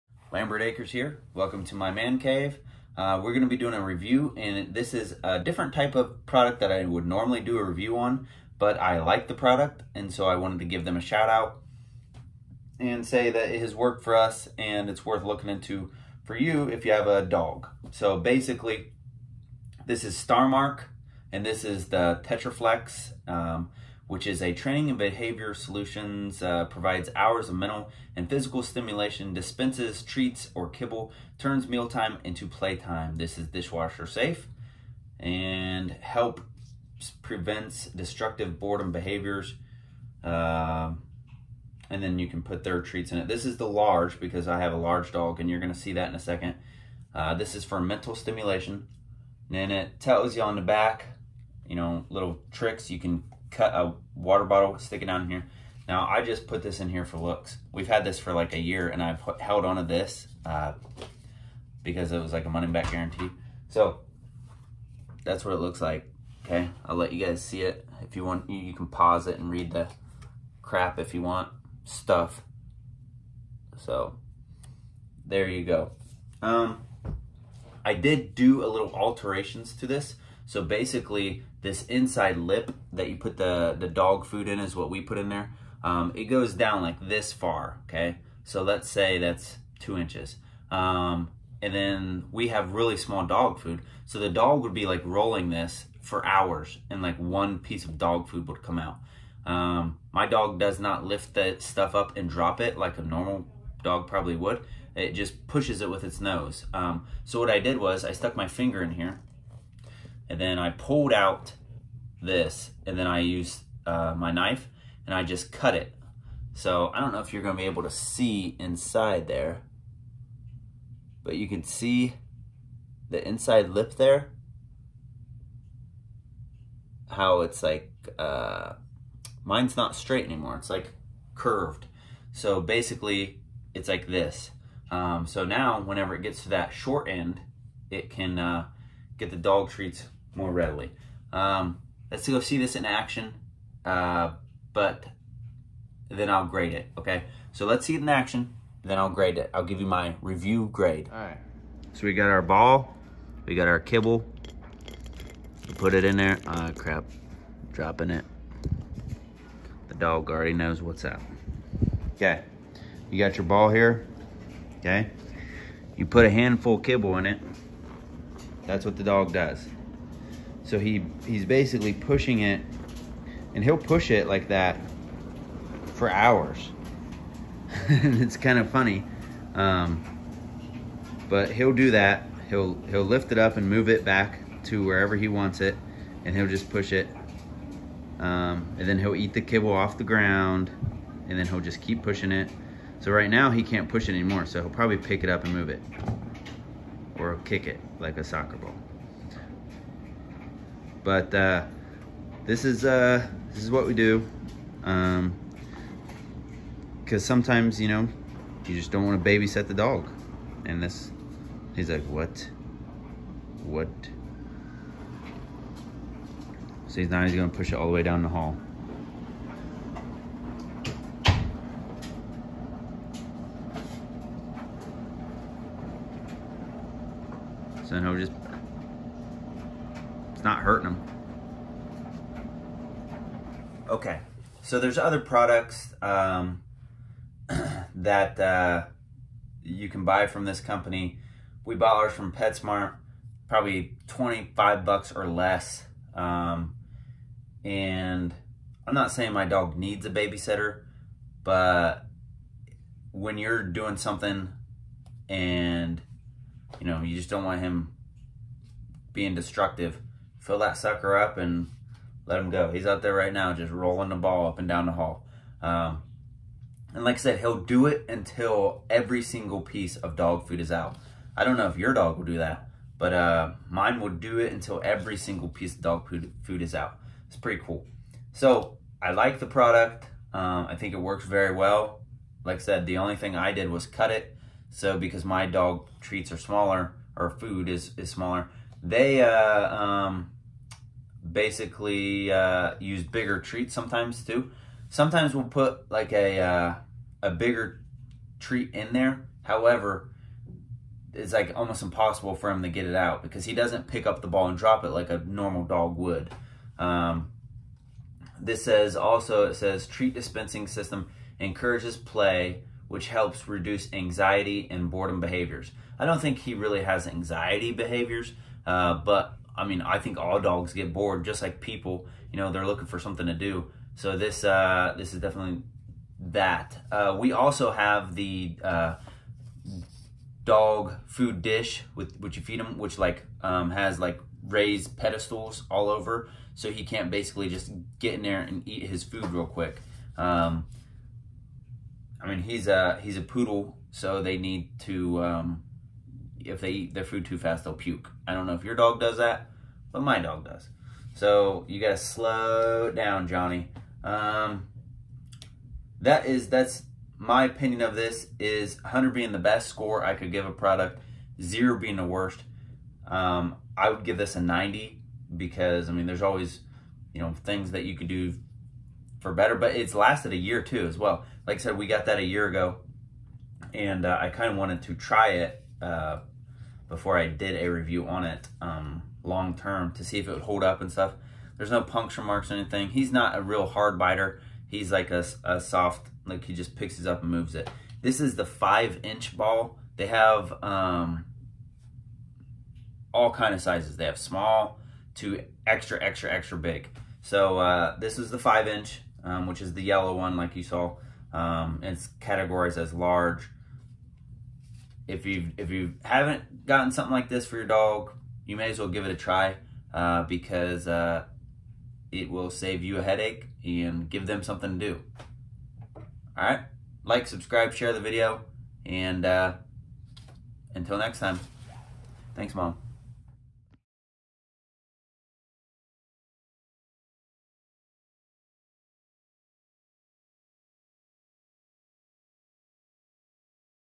Starmark Treat Dispenser Review
I bought this for our dog and made this video to help other people who may be thinking about buying this product or others like it.  Thanks and God bless.